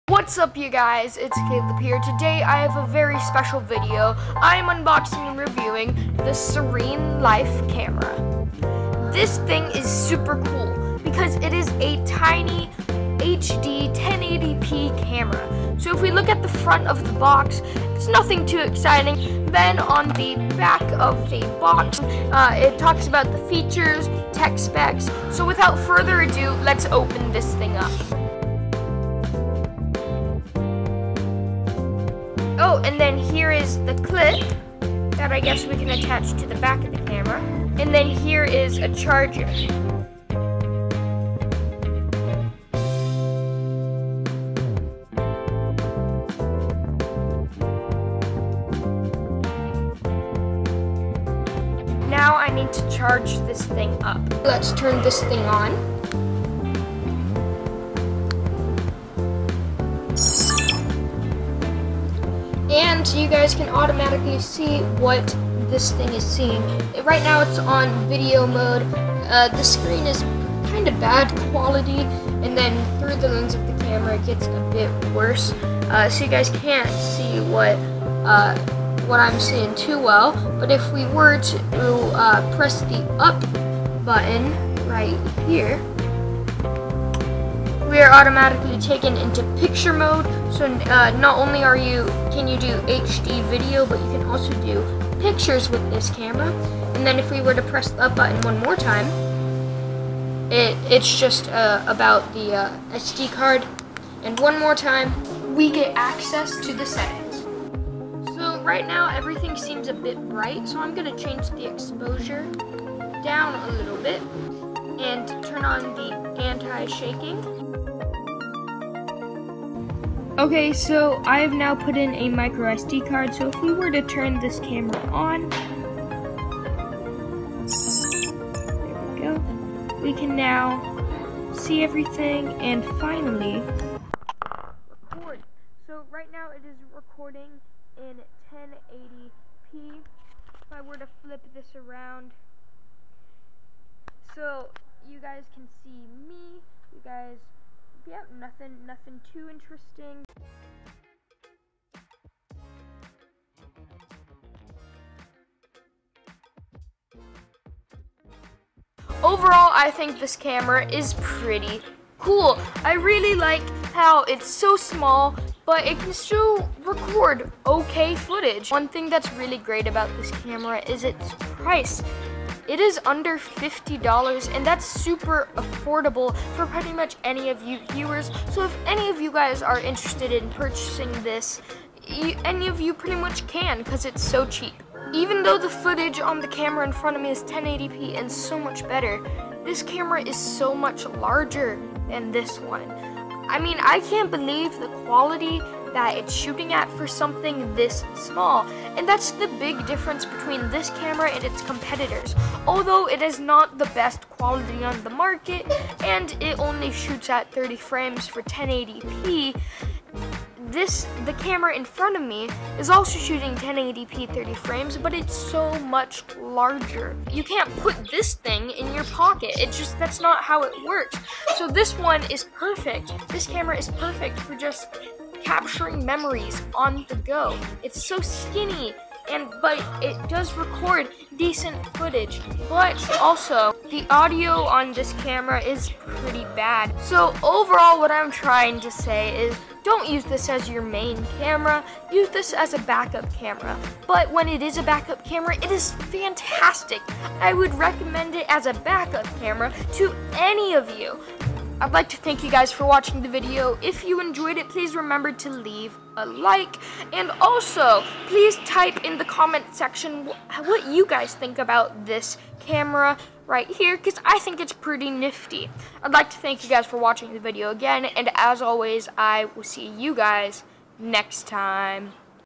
TINY Camera?!? SereneLife Camera Unboxing And Review
Stuff used in this video:

Lakasara Camera

Sony HDR AS-300

64 Gigabyte Micro SD Card and Adapter

Grip
I hope you enjoyed! This camera is super cool, but I would recommend it as a backup camera. Remember to comment questions  you have for the 250 subscriber Q and A!